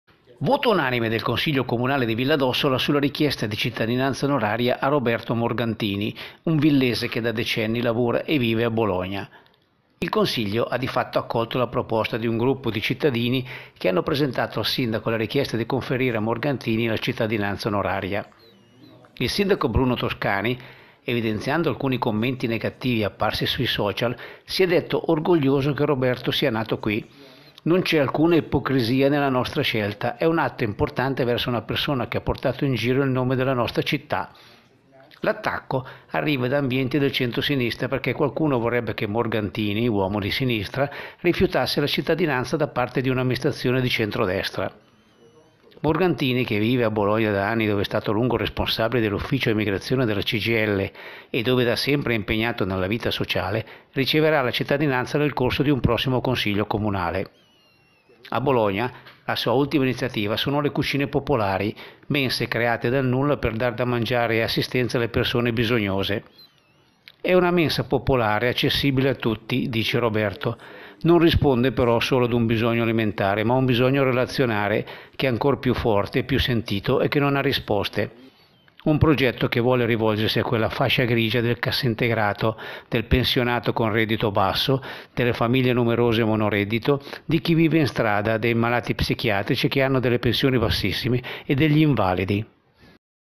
Roberto Morgantini sarà cittadino onorario di Villadossola
8 ottobre 2018 - Il Consiglio Comunale di Villa ha approvato all'unanimità. Toscani: "Atto importante verso una persona che ha portato in giro il nome della nostra città"